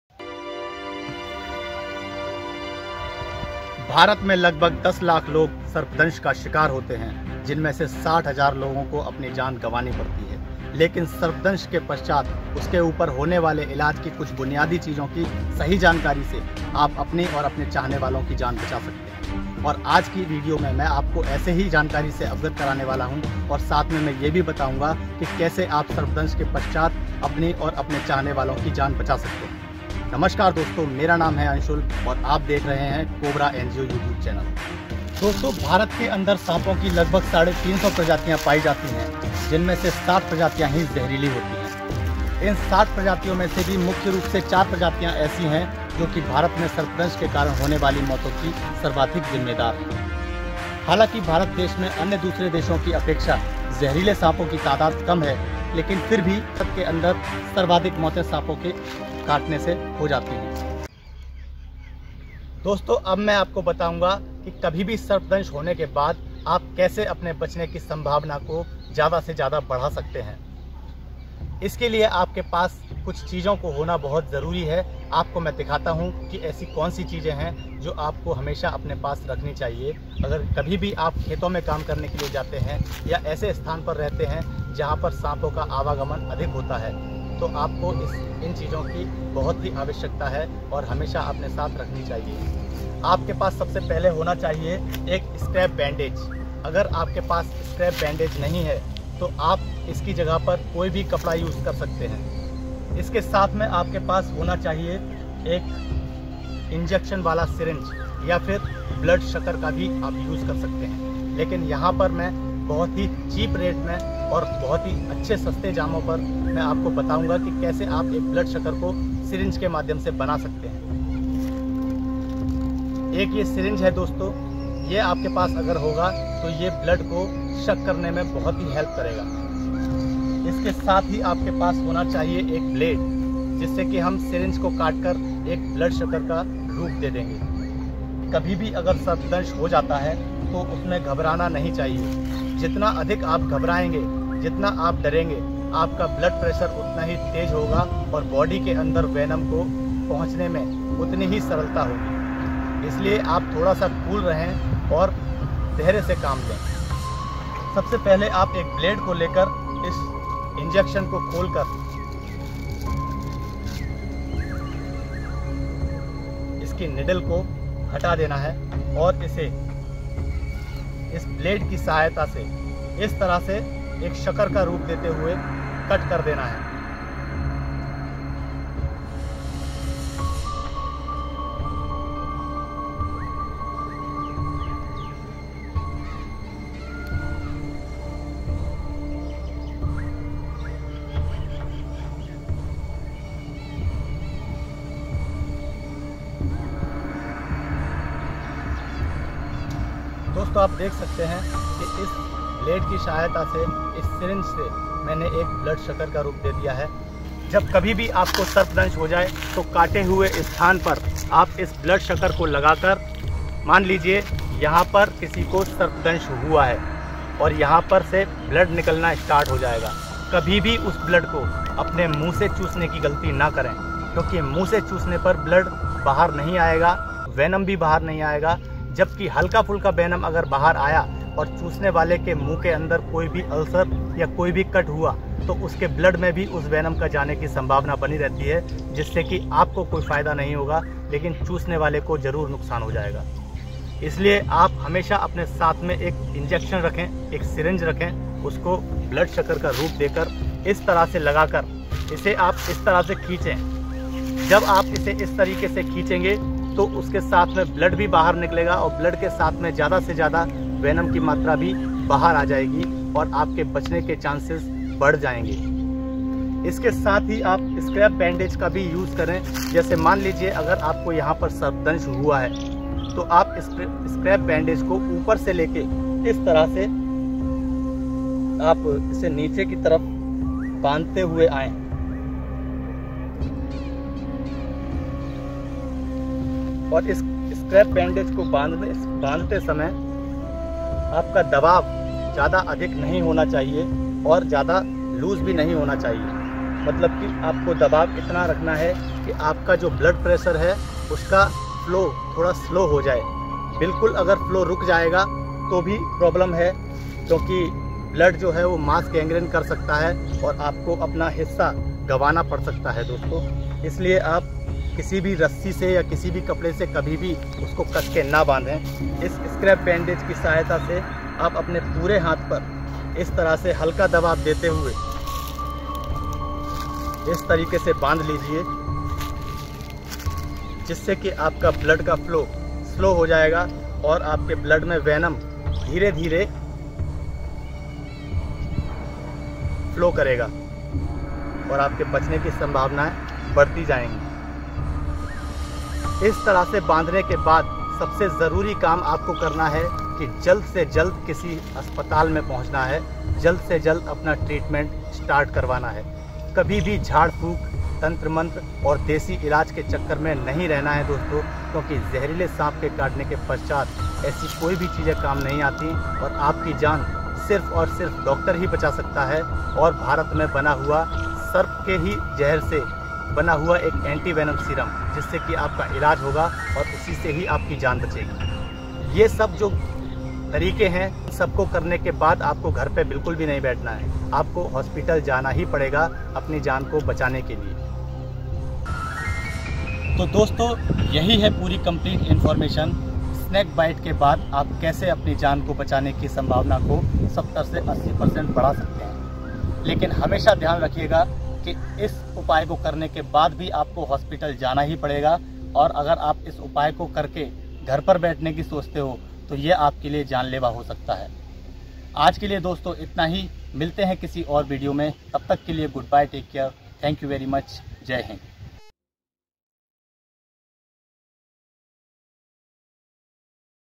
Snake bite treatment|| सर्पदंश का इलाज़|| BY COBRA NGO
HELLO FRIENDS,
                                                          *WARNING*
SNAKE RESCUE का काम एक प्रशिक्षित एवं निपुण व्यक्ति द्वारा किया जाता है आप इसे दोहराने का प्रयास न करें | ऐसा करना आपके लिए जानलेवा हो सकता है |
इस विडियो में हमने सर्पदंश होने पर प्राथमिक उपचार के विषय में जानकारी दी है सर्पदंश के तुरंत पश्चात आप यह उपाय अपनाकर अपनी या अपने चाहने वालों की जान बचाने की संभावना को अधिक बढ़ा सकते है परंतु यह सभी उपाय करने के पश्चात भी आपको अस्पताल अवश्य ही जाना होगा | यदि आप यह समझते हैं की इन उपाय के पश्चात आप घर में ही रुक जाएँ और आपकी जान बच जाएगी तो अप गलती कर रहे है ऐसा करना आपके लिए जानलेवा हो सकता है |
SOME OTHER VIDEOS LINK YOU MAY LIKE THIS:

3. हर समय काटने वाला बहुत गुस्सैल साँप || (COMMON WOLF SNAKE) || ANSHUL DEEP SHAH | BY COBRA NGO